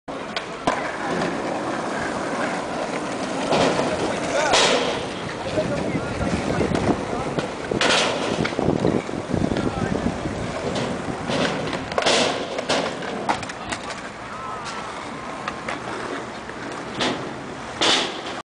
Edi vs Anarhie Resita Skate Contest 2007
Edi din DTS (era mai incepator atunci,acum a luat premii pe la contesturi :D ) vs Anarhie (care era sponsorizat de Bones atunci) la Contestul de la Resita din 5 Mai 2007